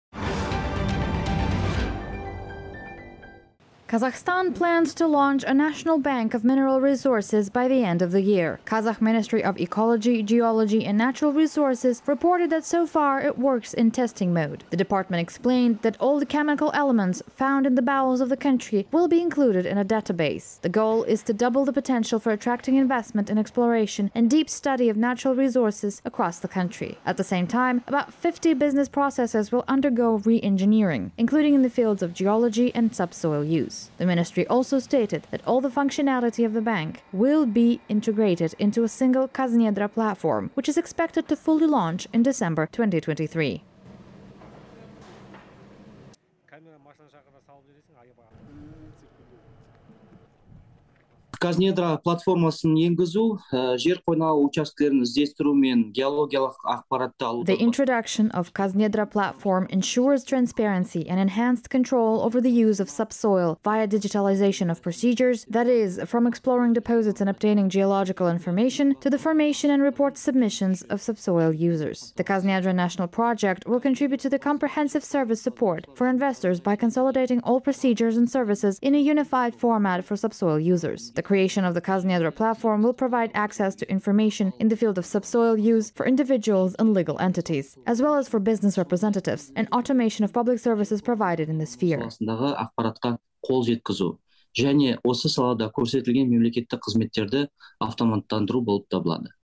Kazakhstan plans to launch National Bank of Mineral Resources in testing mode
Kazakhstan plans to launch a National Bank of Mineral Resources by the end of the year. Kazakh Ministry of Ecology, Geology and Natural Resources reported that so far it works in testing mode. The department explained that all the chemical elements, found in the bowels of the country, will be included in a database. The goal is to double the potential for attracting investment in exploration and deep study of natural resources across the country. At the same time, about 50 business processes will undergo re-engineering, including in the fields of geology and subsoil use. The ministry also stated that all the functionality of the bank will be integrated into a single Kaznedra platform, which is expected to fully launch in December 2023.